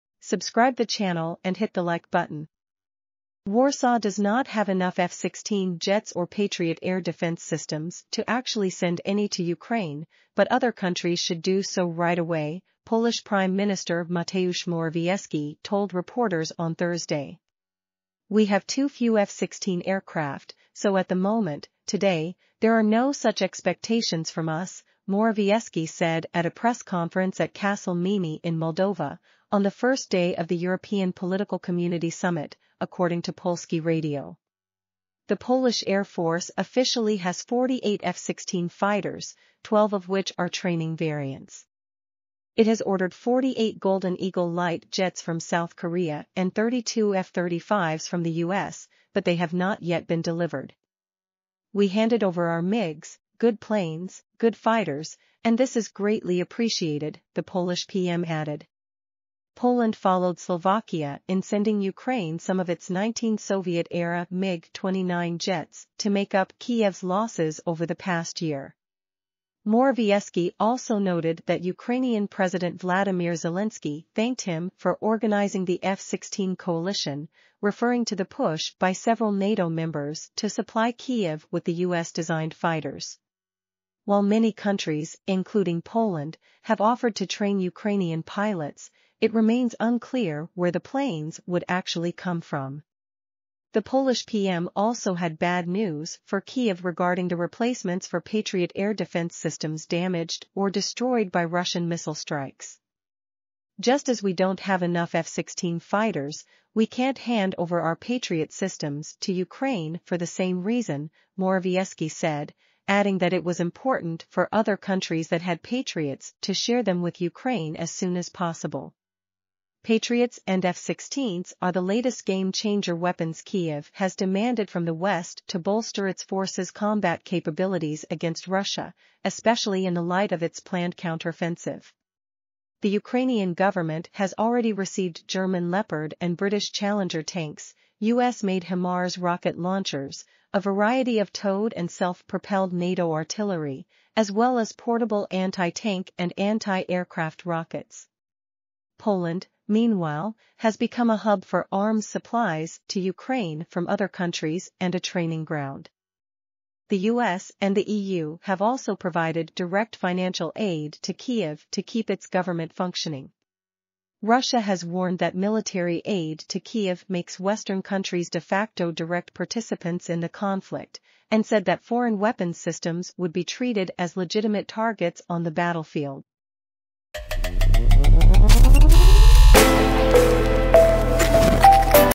Poland won’t supply F-16s to Ukraine
Poland won’t supply F-16s to Ukraine – PM
Mateusz Morawiecki has admitted he doesn’t have the fighter jets to spare#ainewsdaily